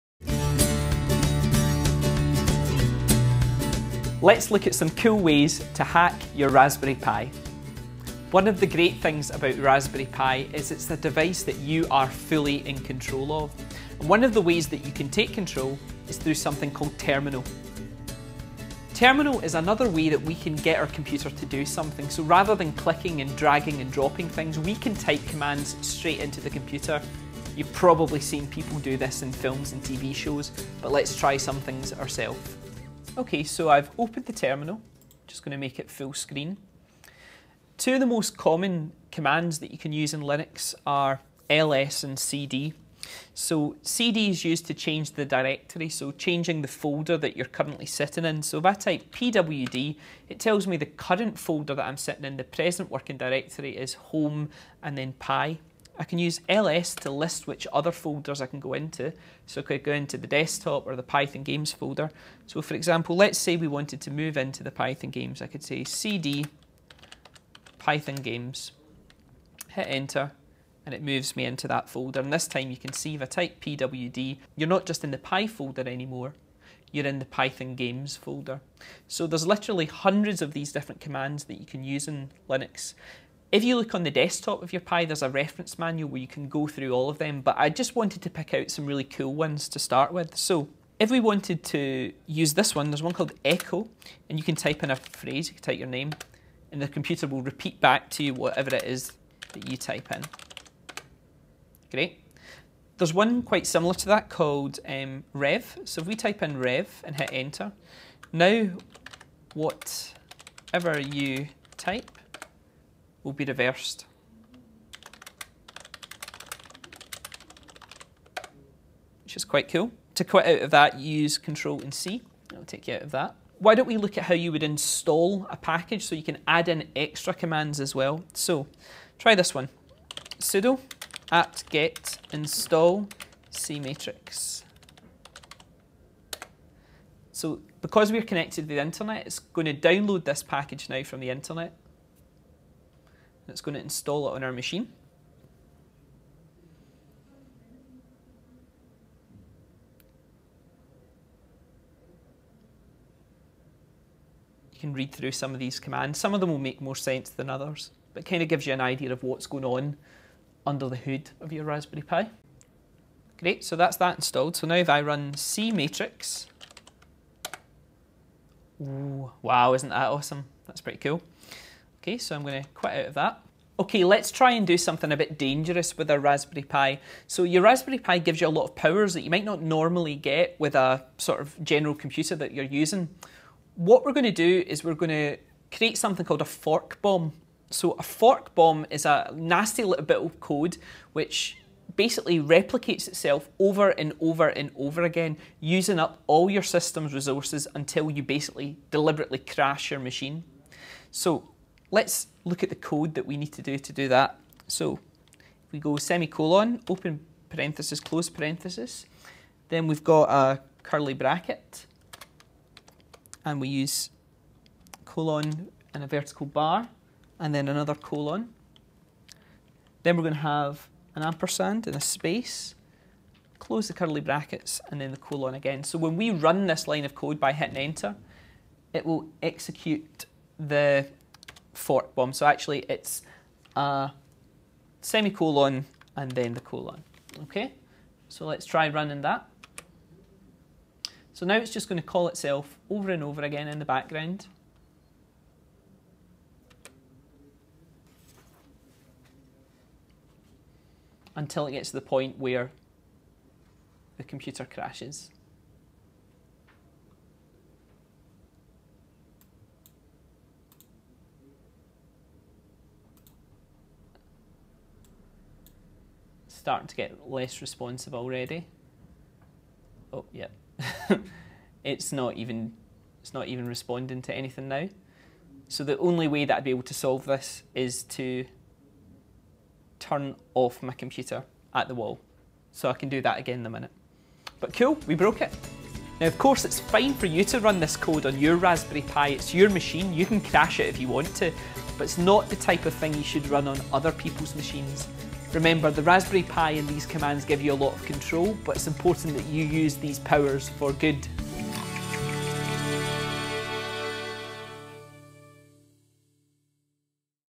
Skyscanner / Young Scot - Get Started
These videos are part of the SkyScanner / Young Scot project to give away Raspberry Pis.

In this video we have a look at some ways to hack your Raspberry Pi.